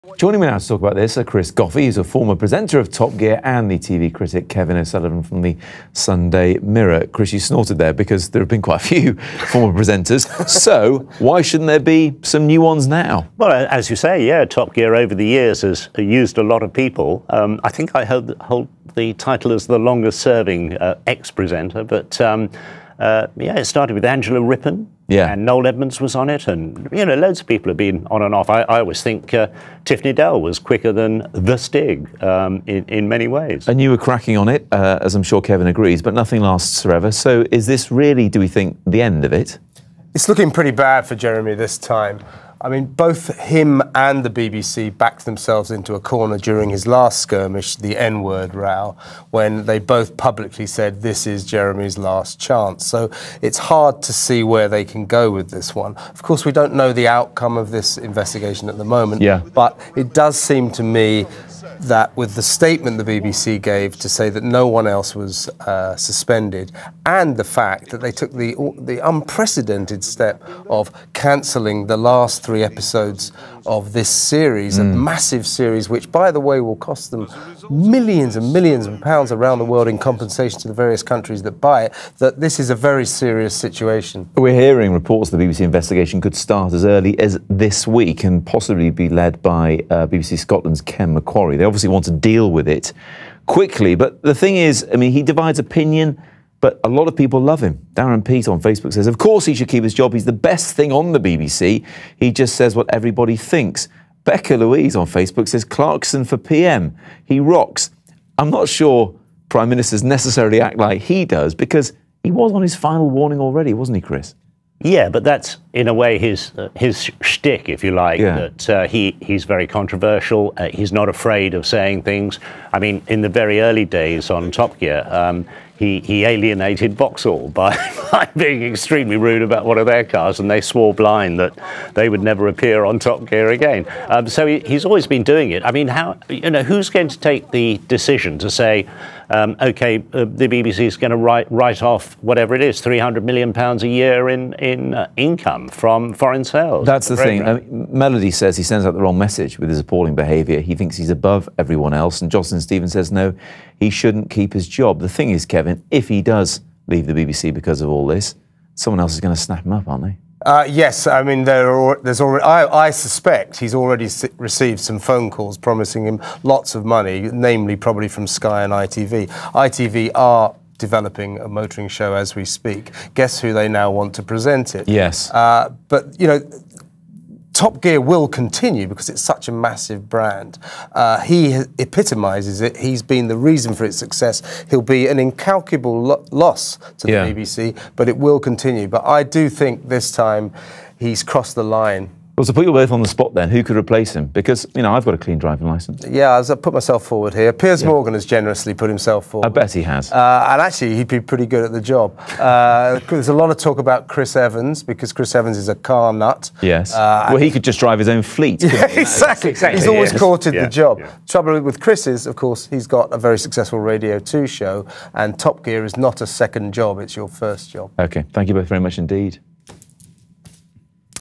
Is Clarkson's career hanging in the balance?
Chris Goffey, a former presenter of Top Gear and TV Critic Kevin O'Sullivan from the Sunday Mirror join Matt Barbett to discuss the latest on Jeremy Clarkson's suspension.